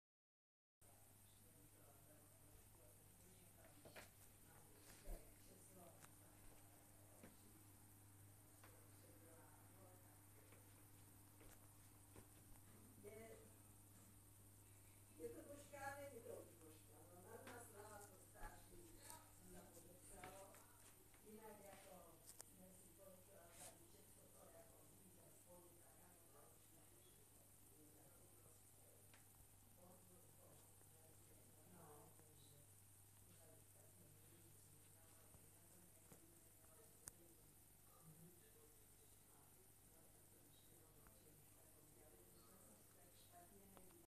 Forest edge II - Scapers Tank Contest
Nádrž, která byla vytvořena pro 2. kolo soutěže Scapers Tank Contest